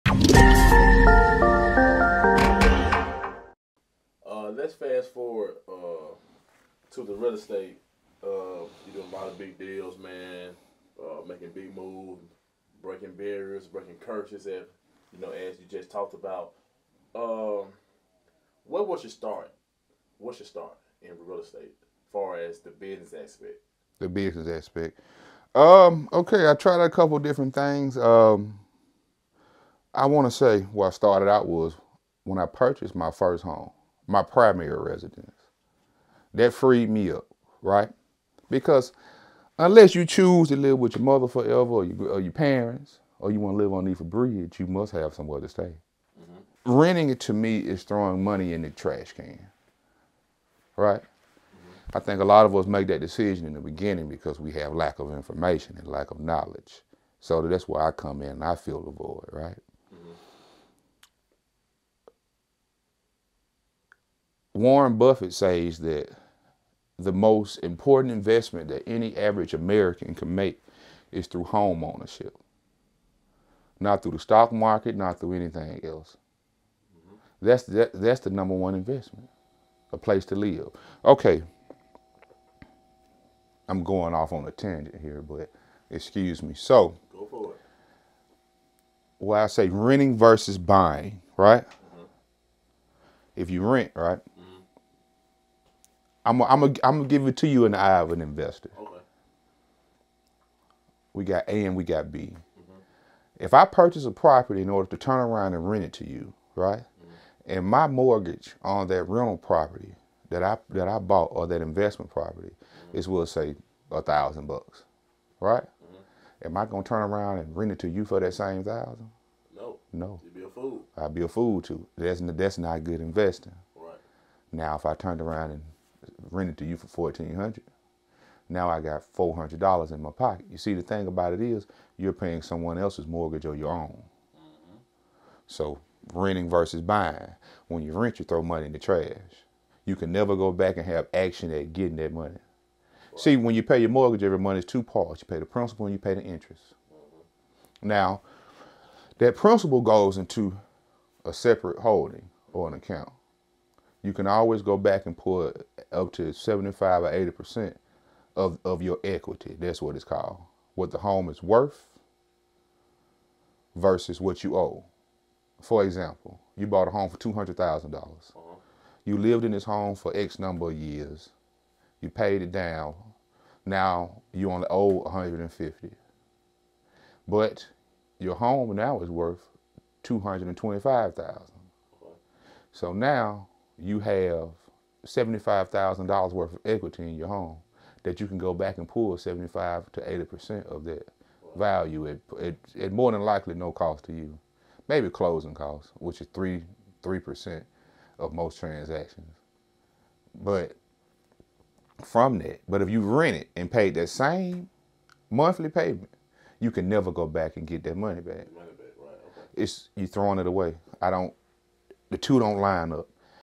Jay Hill -Top Investor talks about stop throwing Money in the trash!! part 4
Alabama Top investor Jay Hill talks about his life and real-estate.